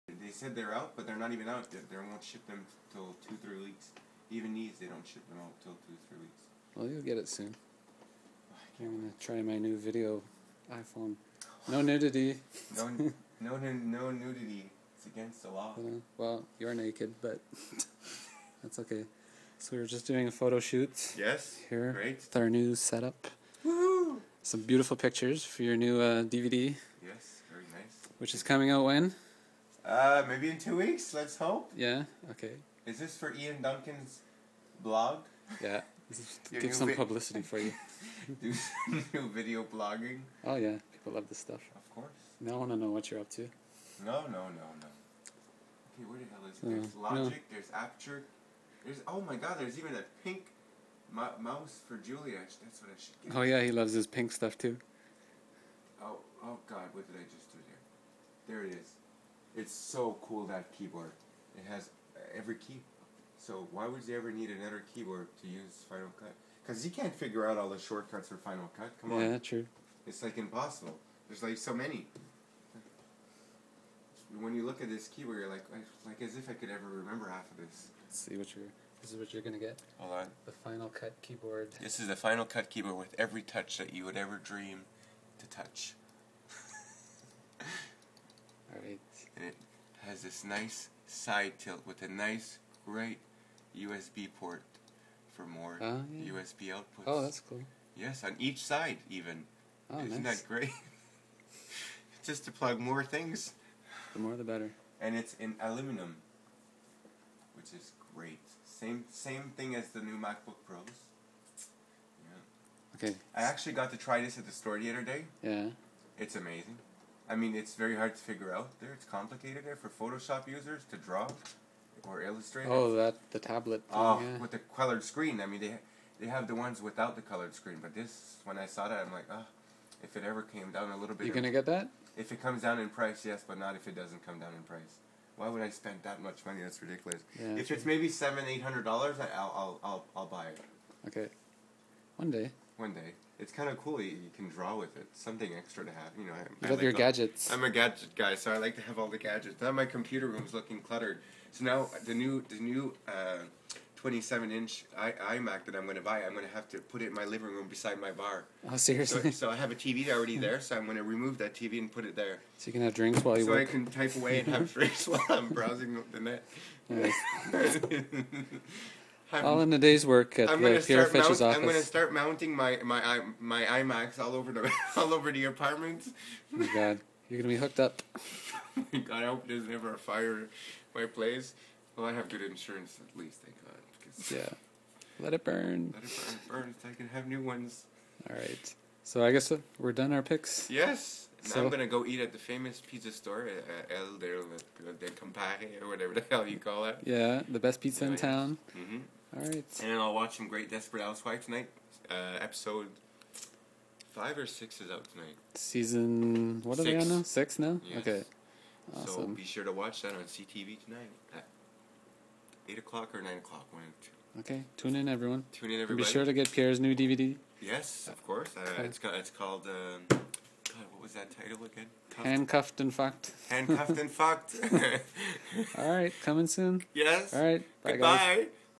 Pierre Fitch at Ian Duncan's Place for a Photo Shoot
Pierre came over today so I could do the cover photo for his new DVD coming out in a couple of weeks. We were trying to stick to a serious tough guy look but we couldn't stop laughing the whole time. But it's OK because we had fun and I got some amazing pics! :D